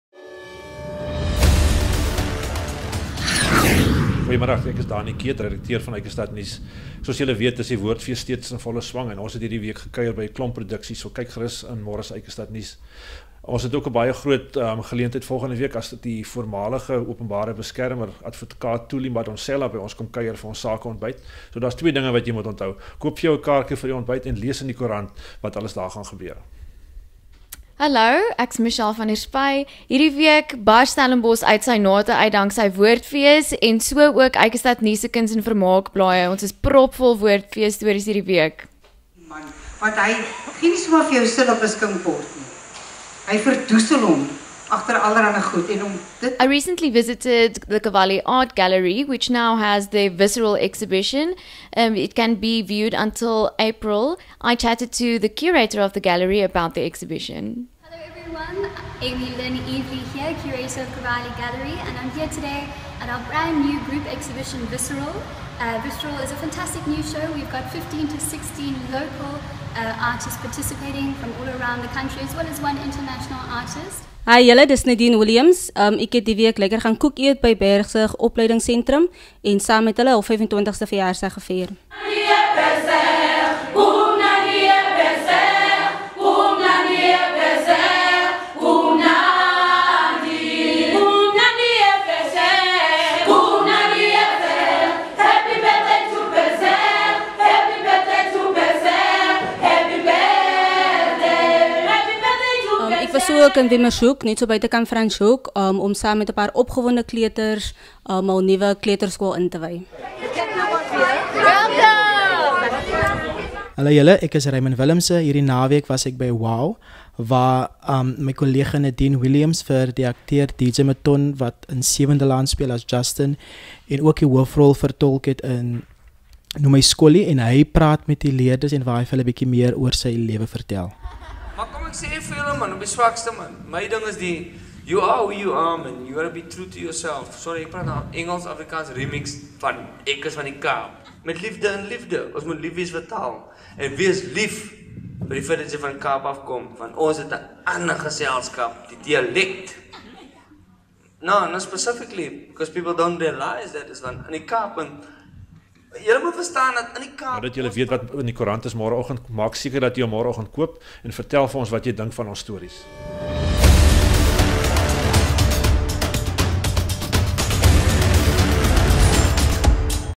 Eikestad eNuus Bulletin 8 Maart 2017
Hierdie week bring ons weer die nuus vars uit Stellenbosch.  Die Woordfees is in volle swang, met heelwat produksies wat die week plaasvind.  Advokaat Thuli Madonsela gaan volgende week die Eikestad se sakemanne toespreek, lees meer daaroor in die week se EikestadNuus...